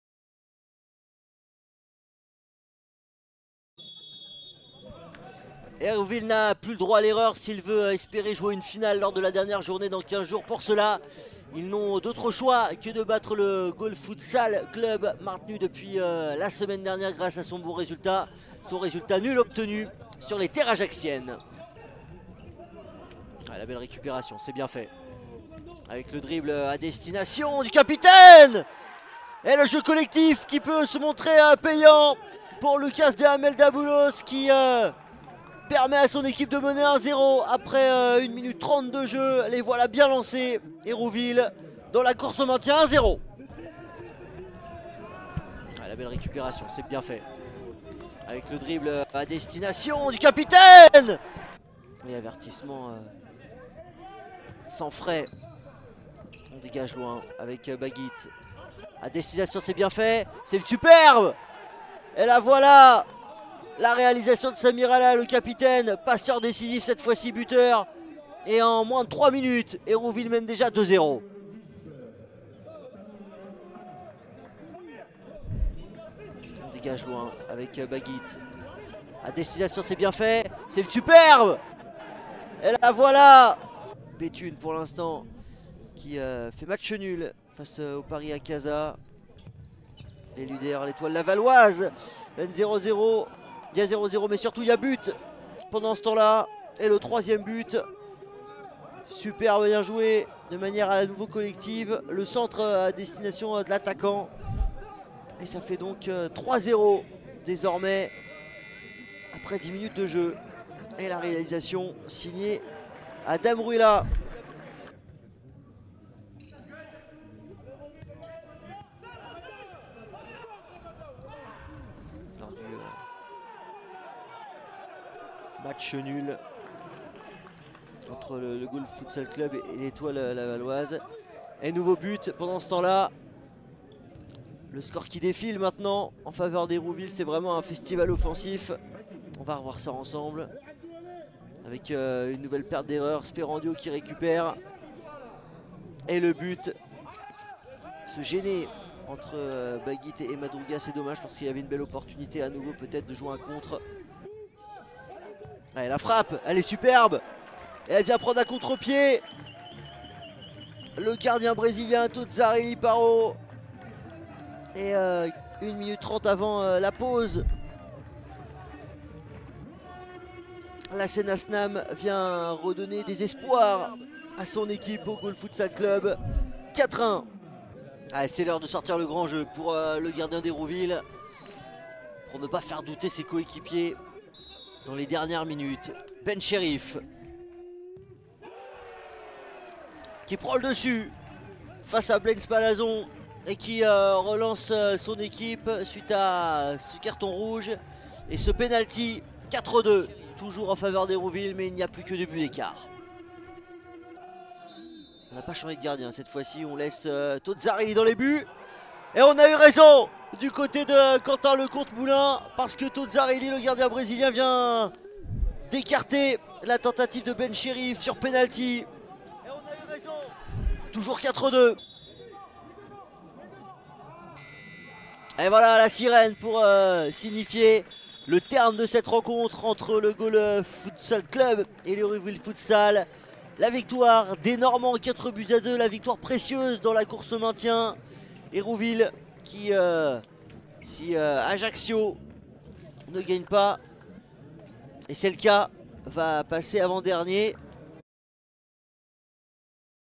J21 : Goal Futsal - Hérouville Futsal (2-4)
Découvrez l'ensemble des buts inscrits lors de ce match comptant pour la 21e journée du Championnat.